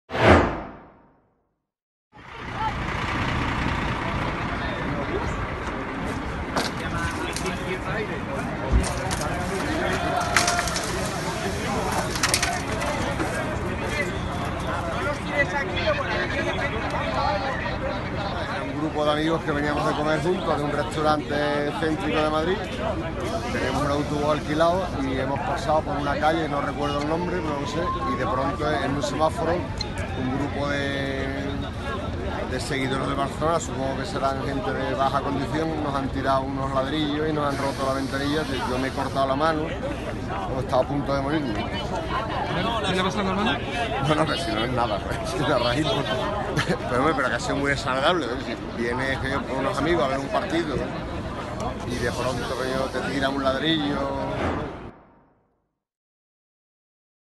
Final Copa Rey 2018: Un Autobús Sevillista Apedreado Antes de la Final de Copa ante el Barcelona
Un autobús de la afición del Sevilla sufrió un apedreamiento antes de la final de Copa ante el Barcelona en el Wanda